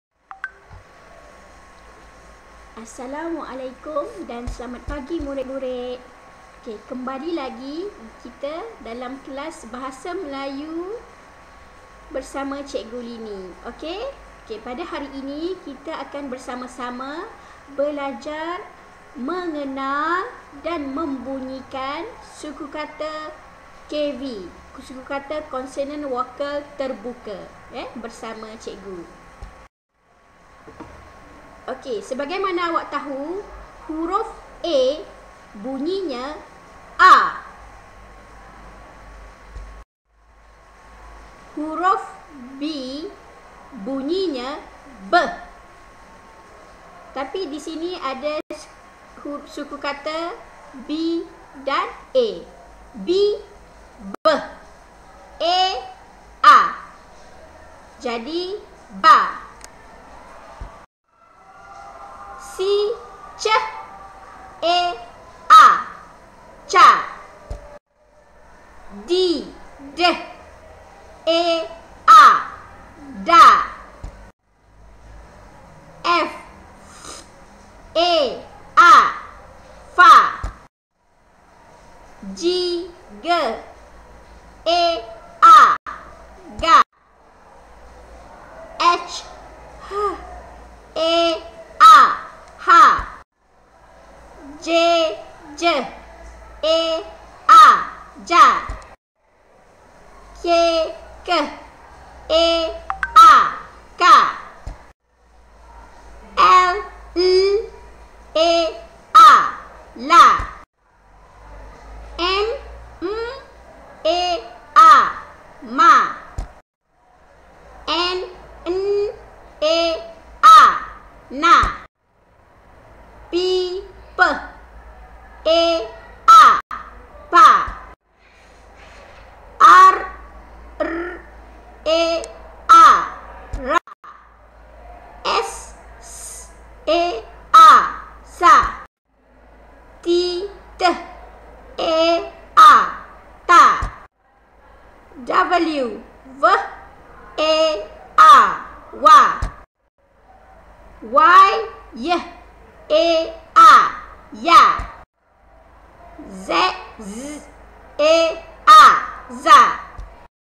SUKUKATA KV terbuka bersama huruf vokal A
Pada akhir pengajaran, murid dapat :
1) Mengeja sukukata KV
2) Membunyikan sukukata KV